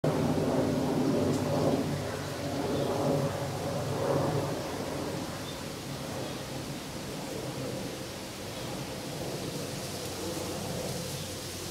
Cetti's Warbler at nest
Cetti's Warbler feeding and nest-cleaning activity, filmed May 2012 at S'Albufera, Mallorca.